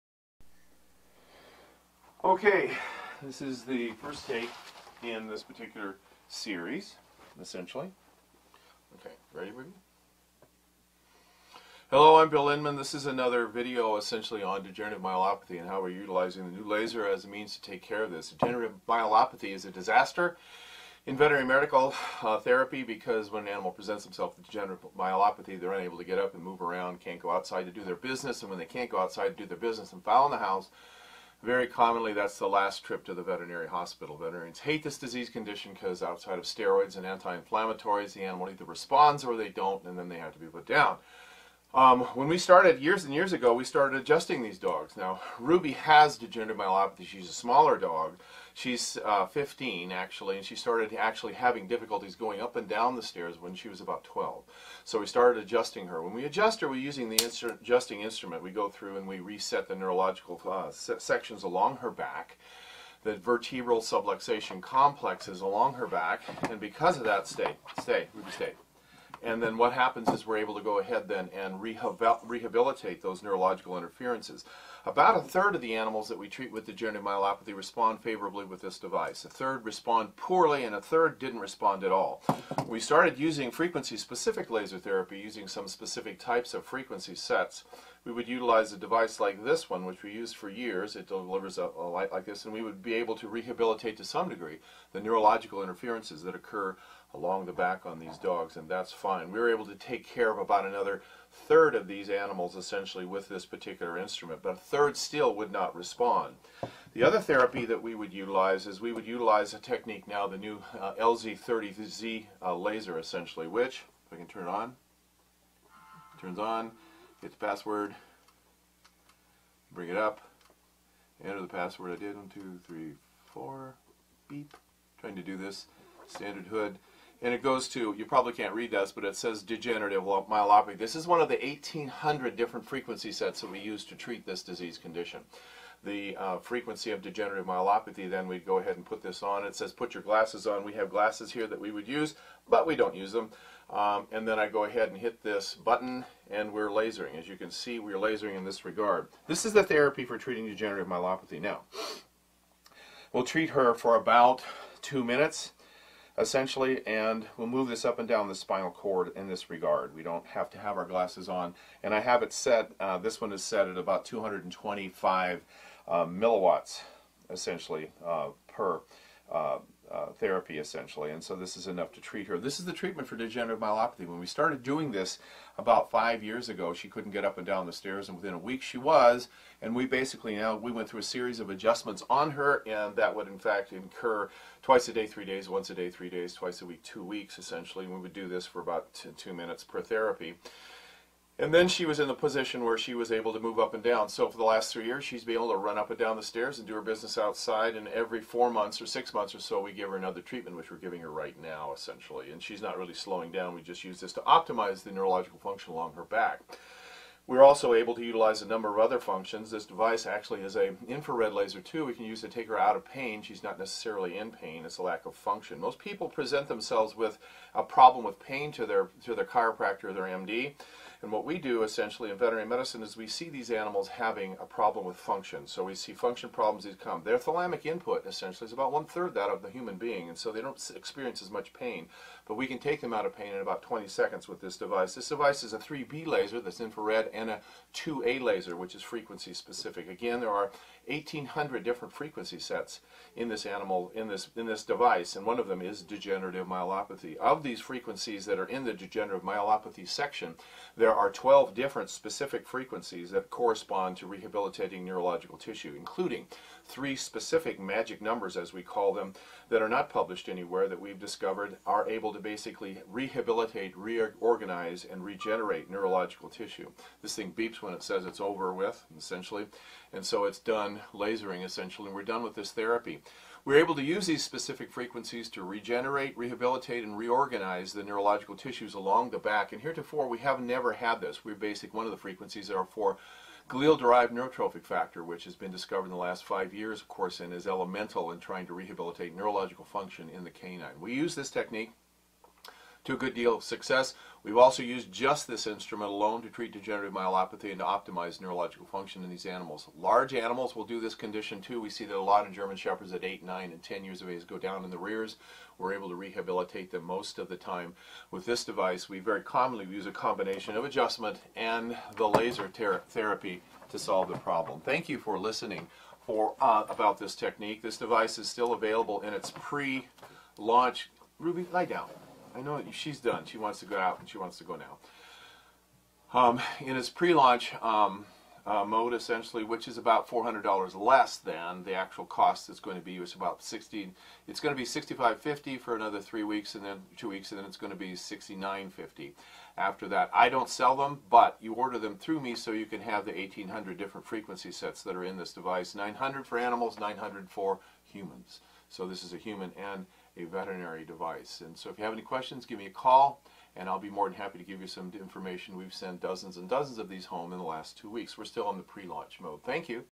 Degenerative Myelopathy Treatment Using the LZ30-z Laser
The Condition  Of K-9 Degenerative Myelopathy (DM) is discussed as to cause, diagnosis and treatment using several methods and focousing on the appication Of Frequency-Specific Low Level Laser Therapy using the new Avant LZ30 Laser system with pre=programmed protocols.